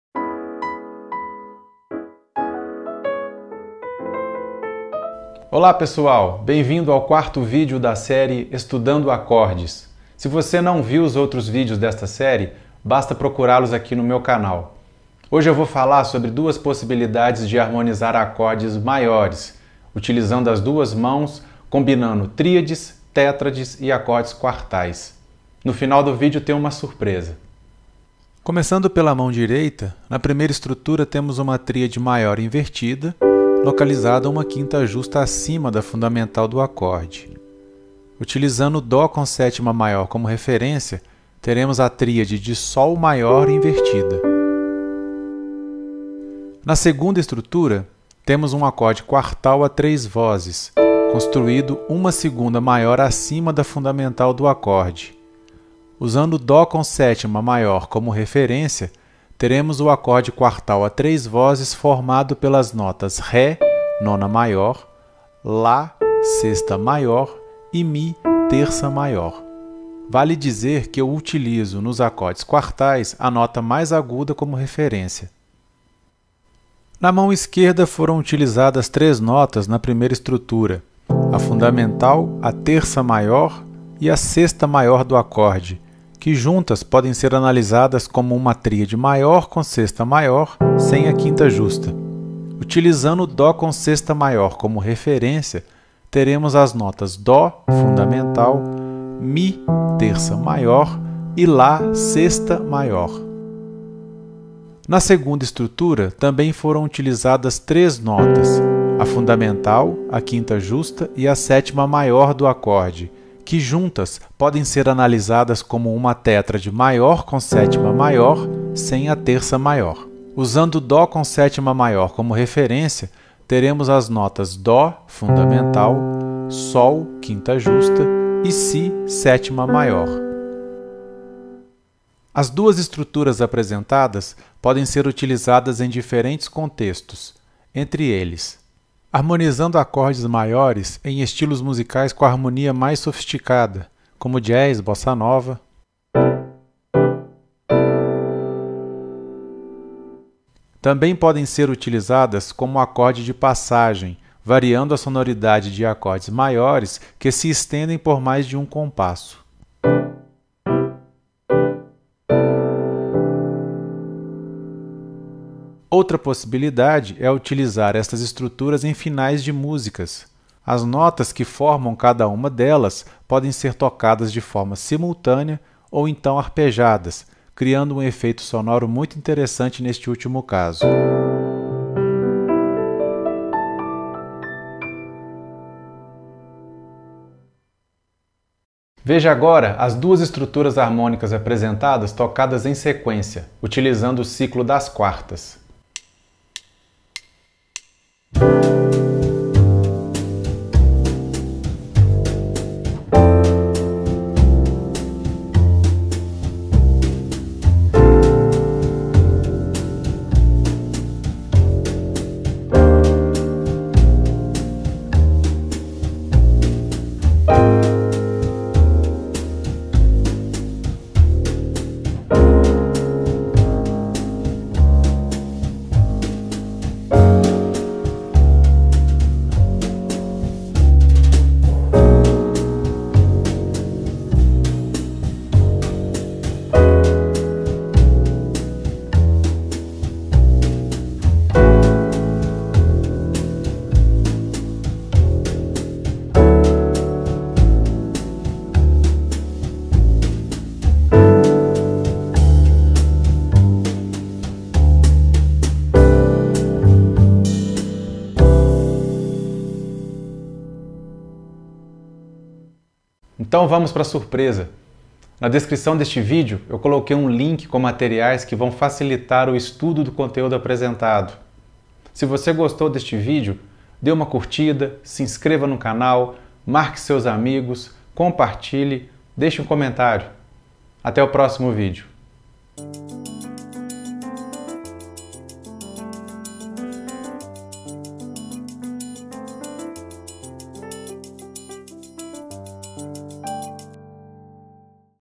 Estudando acordes #4 - Acorde Maior - Piano Popular
Bem-vindo ao vídeo #4 da série "Estudando Acordes".

Neste vídeo tutorial sobre harmonia no piano e teclado, o músico e educador Felipe Moreira apresenta duas possibilidades de harmonizar acordes maiores, utilizando as duas mãos, combinando tríades, tétrades e acordes quartais.

Também são abordados alguns aspectos teóricos e práticos destas estruturas na harmonia na música popular.

O conteúdo deste vídeo é direcionado para pianistas e tecladistas de nível intermediário, podendo também ser utilizado por professores, regentes e músicos que tenham conhecimentos básicos de harmonia no piano ou teclado.

Isso ajuda na divulgação desses conteúdos para pessoas que tem interesse nos assuntos aqui apresentados!

Dessa forma, vamos construindo um canal rico em informações e troca de experiências!

=== Link para download do PDF e mp3 ===